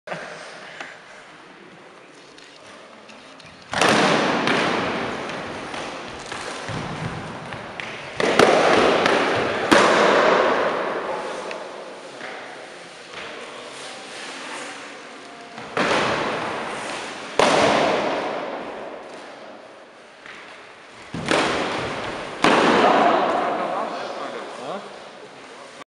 Спорт - меч
Двуручный меч
Радионов Илья - Авениров Николай